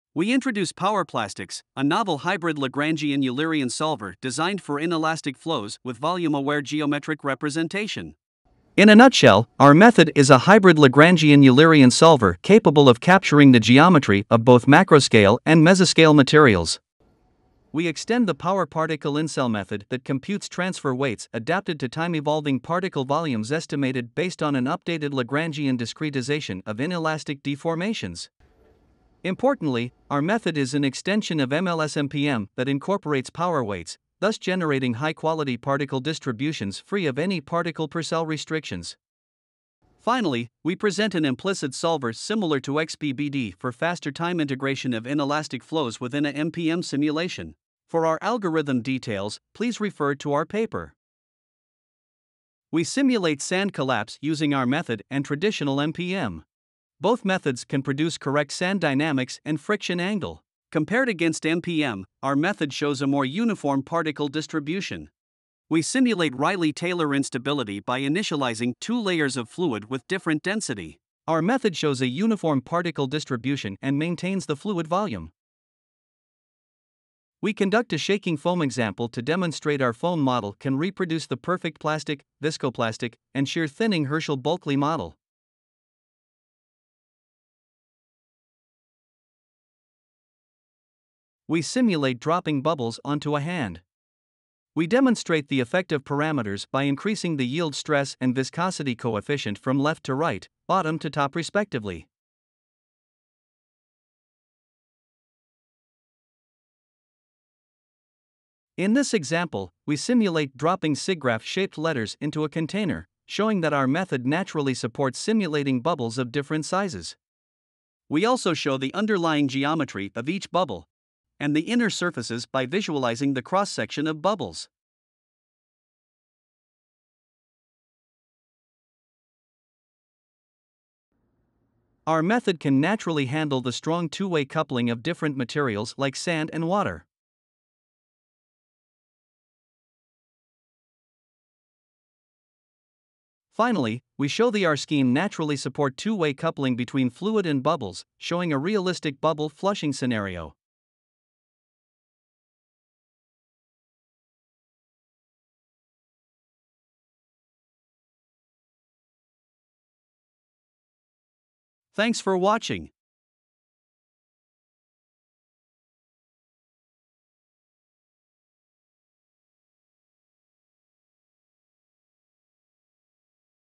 Power Plastics: A Hybrid Lagrangian/Eulerian Solver for Mesoscale Inelastic Flows
We present a novel hybrid Lagrangian/Eulerian method for simulating in-
elastic flows that generates high-quality particle distributions with adaptive
volumes. At its core, our approach integrates an updated Lagrangian time
discretization of continuum mechanics with the Power Particle-In-Cell geo-
metric representation of deformable materials. As a result, we obtain material
points described by optimized density kernels that precisely track the vary-
ing particle volumes both spatially and temporally. For efficient CFL-rate
simulations, we also propose an implicit time integration for our system
using a non-linear Gauss-Seidel solver inspired by X-PBD, viewing Eulerian
nodal velocities as primal variables. We demonstrate the versatility of our
method with simulations of mesoscale bubbles, sands, liquid, and foams.